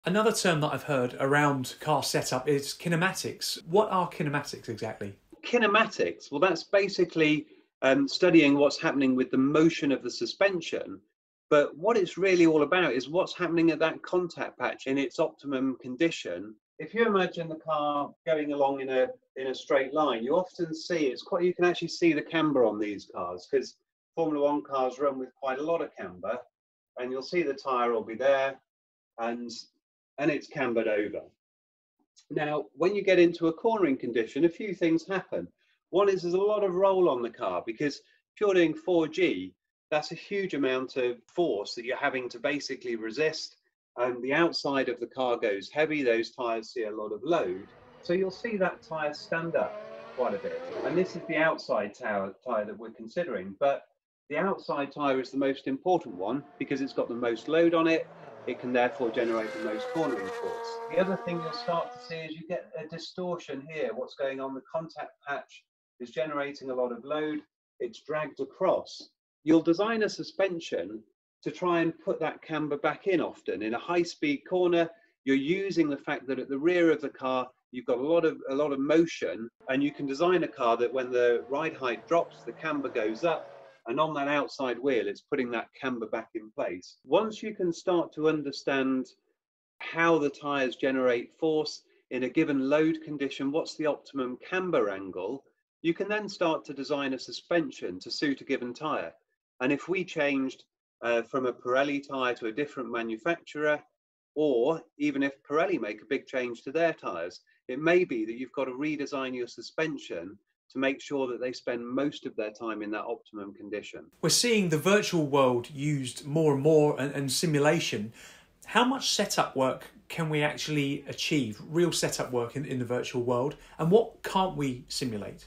Kinematics: How Does Your Car Turn?
We explore car kinematics! Learn about suspension motion, contact patches, and how they affect tire performance, especially under load. We also discuss virtual setups and simulations.